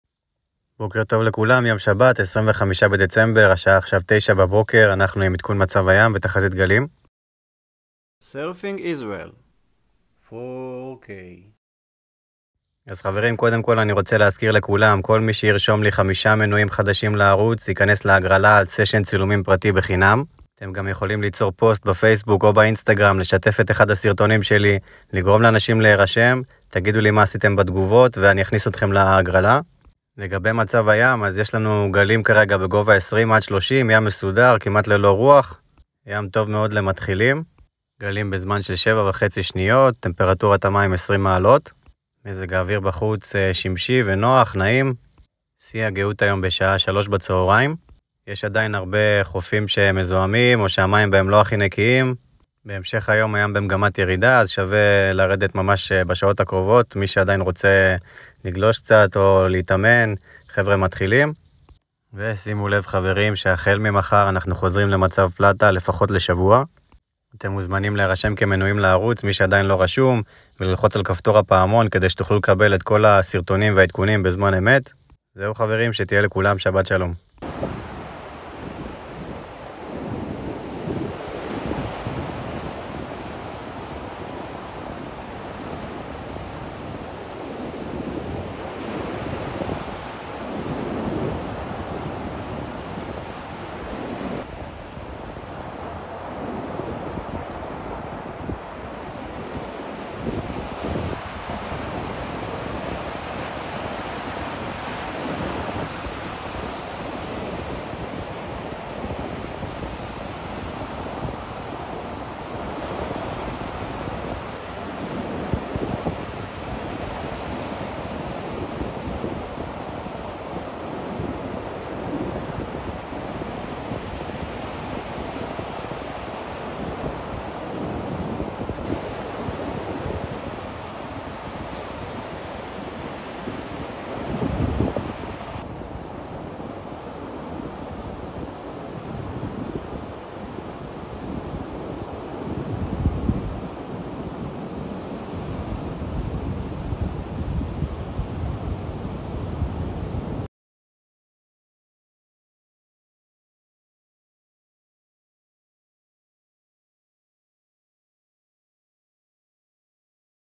25.12.21 ים מסודר למתחילים ! מצב הים תחזית גלים ישראל שבת בוקר Israel Surf Report
מומלצים
‏Our Recommended Videos: